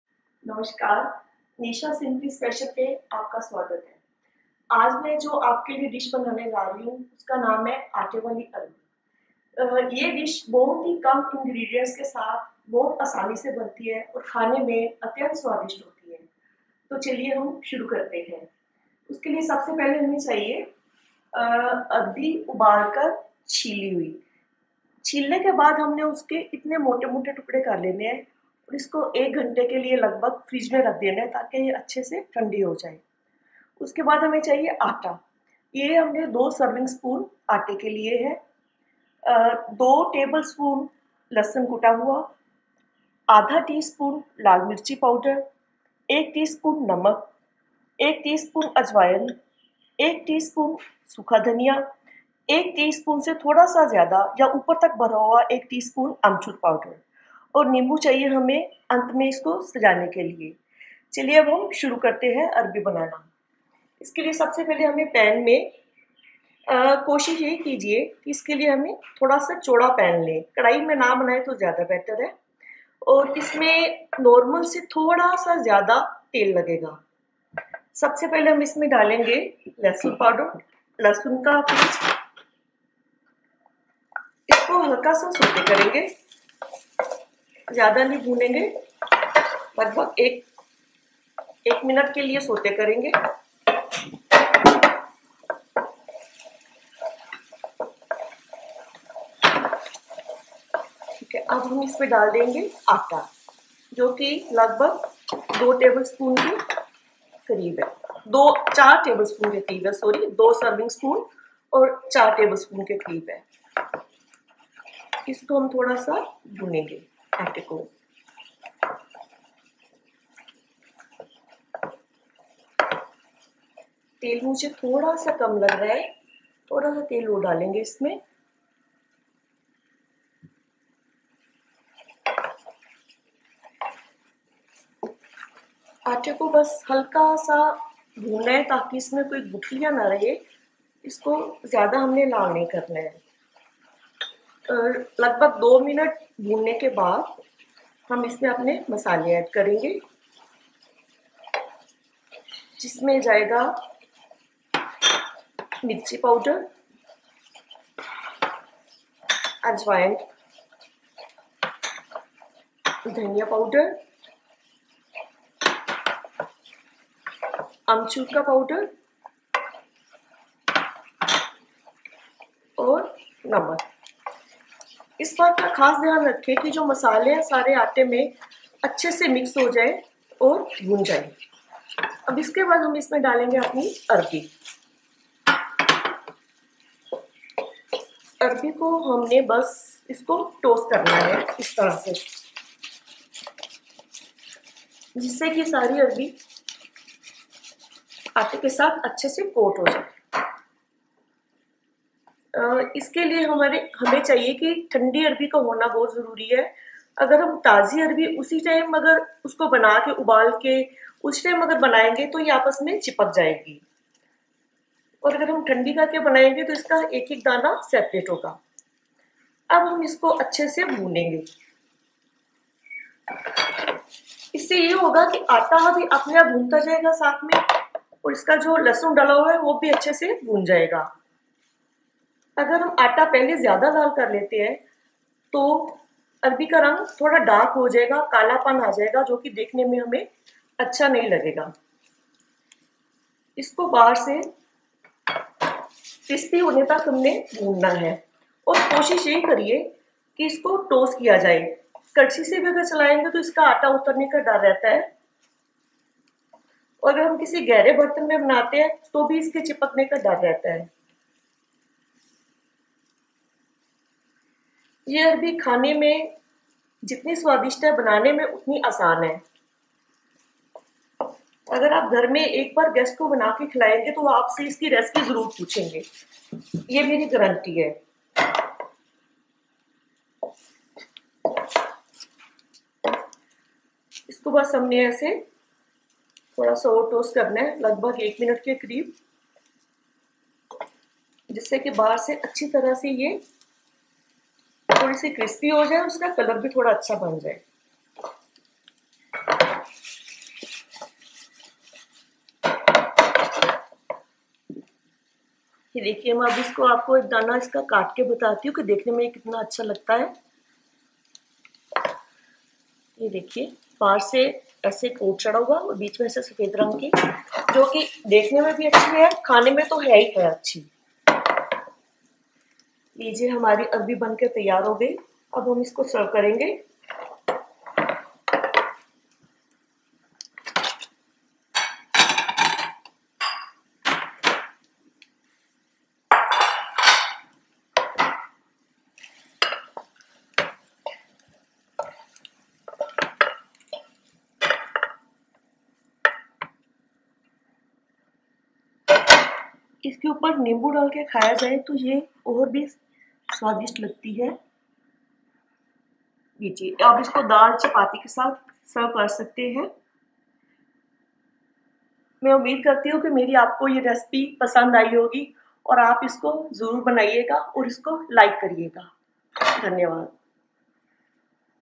Attey Wali Arbi
A delicious dry Arbi recipe that can be accompanied with chapati and dal or served as a snack.

Ingredients
1/2 kg Arbi, boiled and peeled
4tbsp Aata
2 tbsp Garlic, crushed
1 tsp Ajwain
1 tsp Dhaniya Powder
1+1/4 tsp Aamchur powder
1/2 tsp Red chilli powder
Salt to taste
1/3 cup Oil